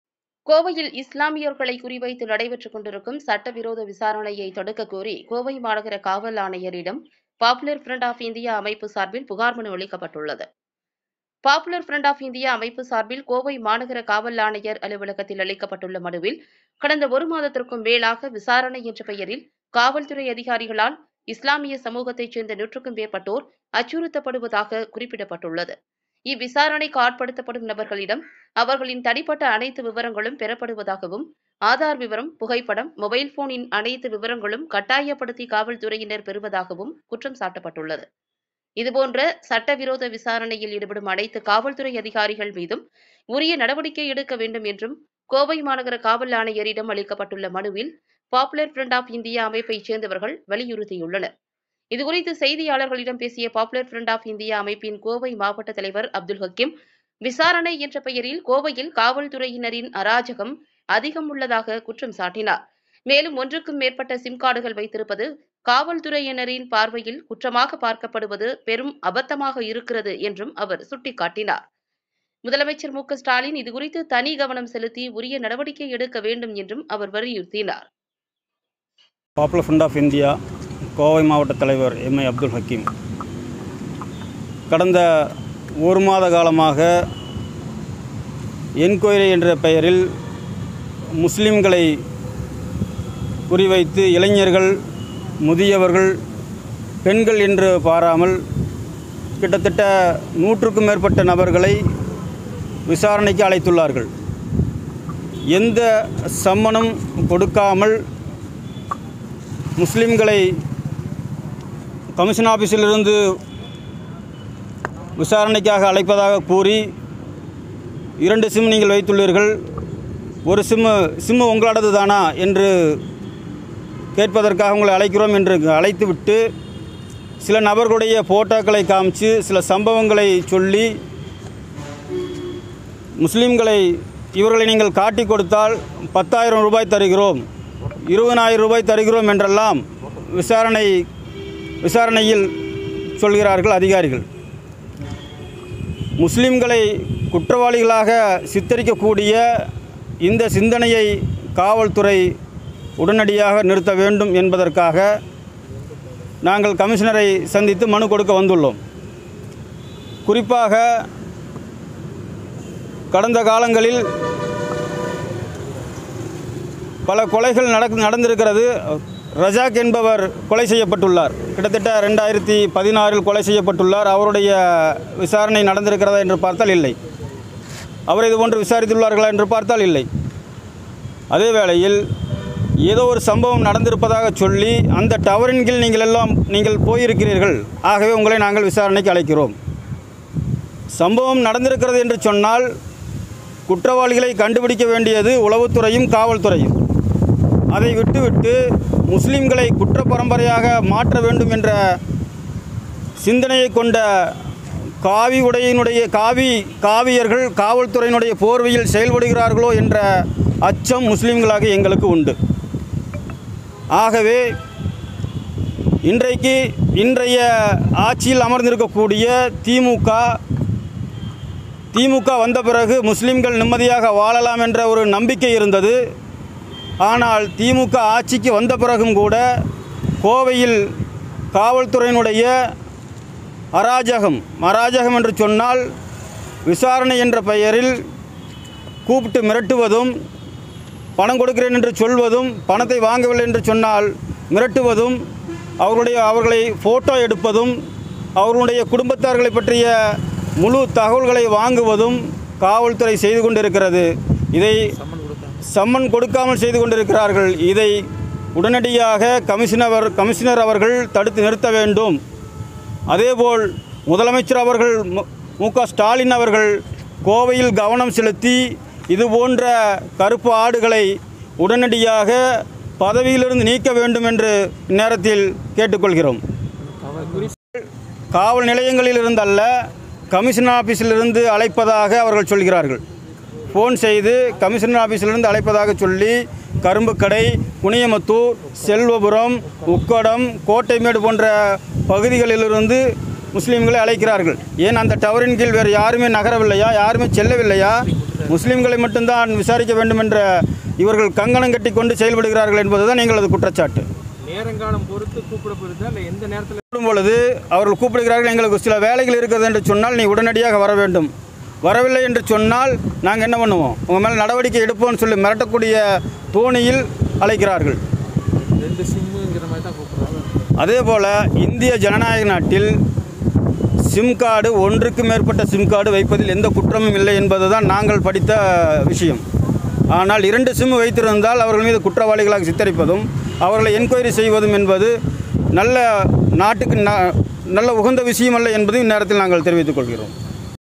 Kovai காவல் ஆணையரிடம் பாப்புலர் ஃப்ரண்ட் ஆஃப் இந்தியா அமைப்பு புகார் மனு
கோவையில் இஸ்லாமியர்களை குறிவைத்து நடைபெற்றுக்கொண்டிருக்கும் சட்டவிரோத விசாரணையை தடுக்கக் கோரி கோவை மாநகர காவல் ஆணையரிடம் பாப்புலர் ஃப்ரண்ட் ஆஃப் இந்தியா அமைப்பு சார்பில் புகார் மனு அளிக்கப்பட்டுள்ளது.